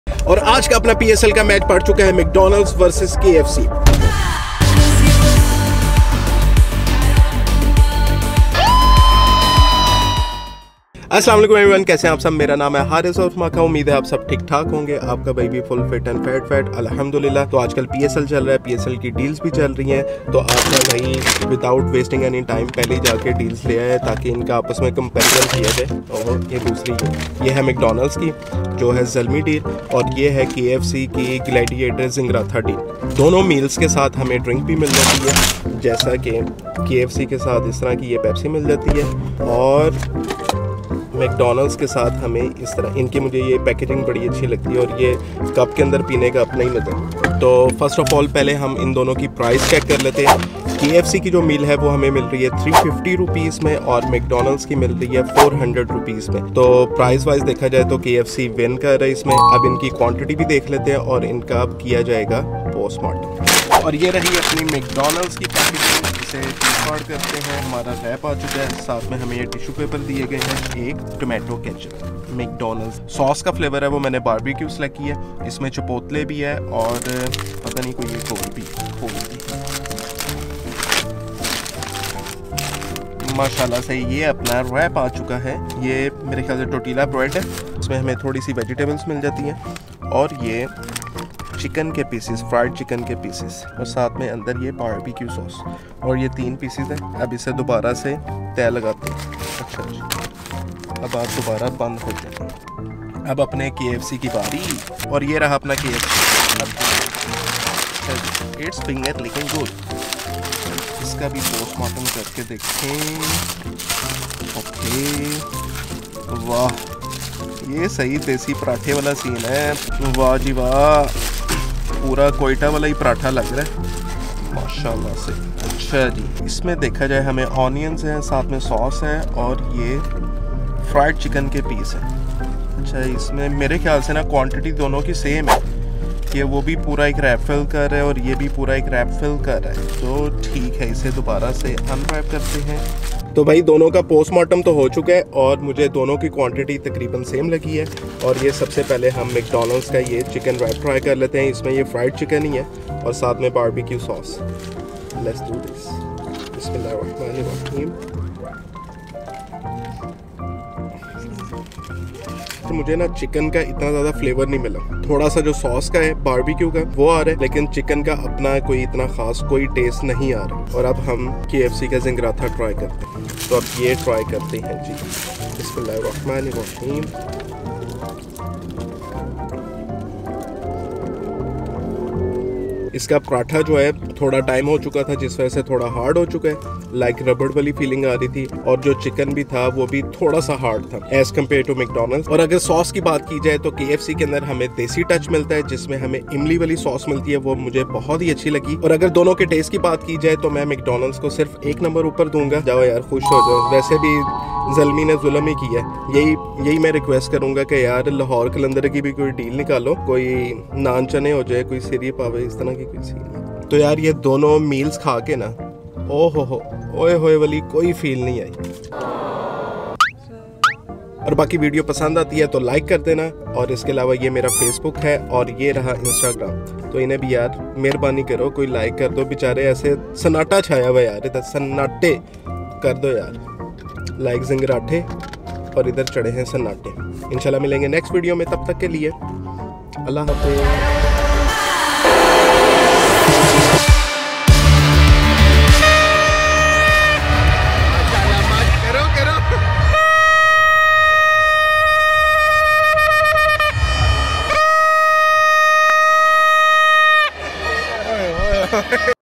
McDonald's vs. KFC? Which is Better? | PSL 2022 Deals Food Review | Mhka Vlog
Hi, in this video I'm going to try different chicken wraps of McDonald's and kfc. i will review them and compare them with their prices, quantity and taste. so lets find out which chicken wrap is better, these are Psl 2022 deals and it's ending soon So go and get yours meal before psl ends. You can save up to 130rs on Macdonald's Zalmi deal or if you are true desi lover then must try the hot and fresh kfc zingratha with full filled of imli cutni. mmmmmmmm.